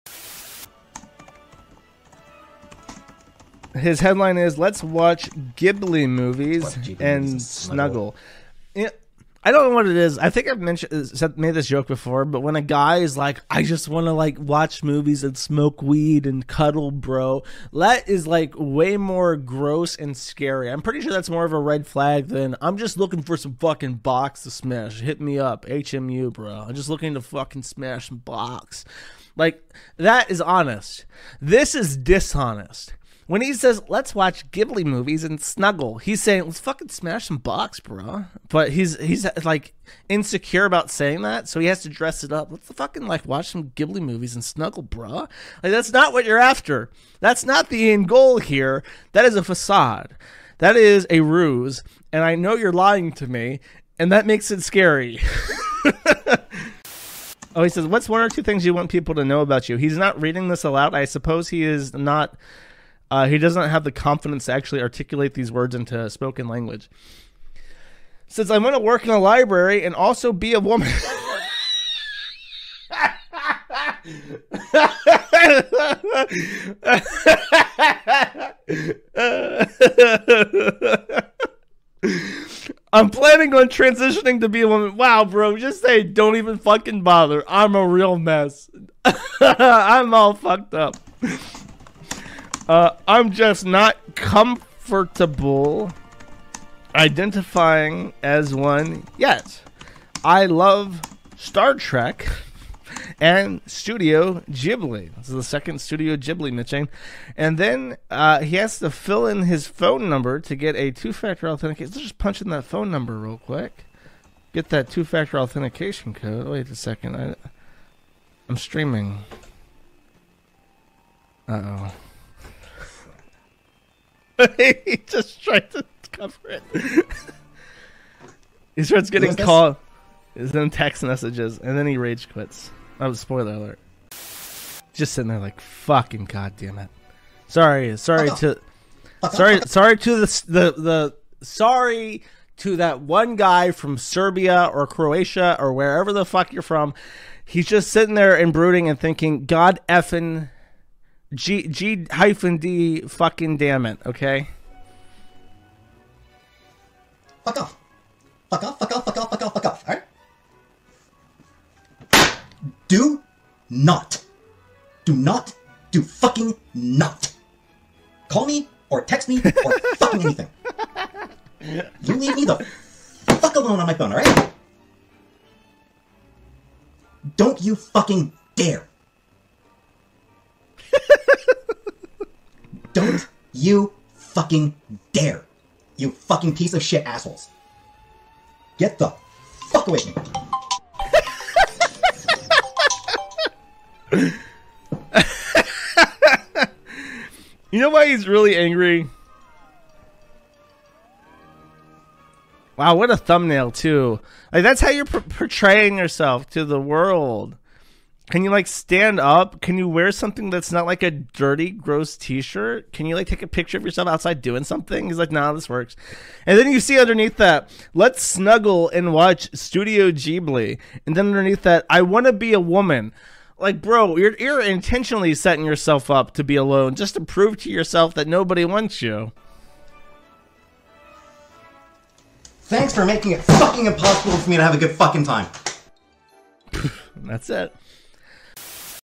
Josh Reacts to Peetz Dating Attempts
Josh Reacts to Peetz doxing himself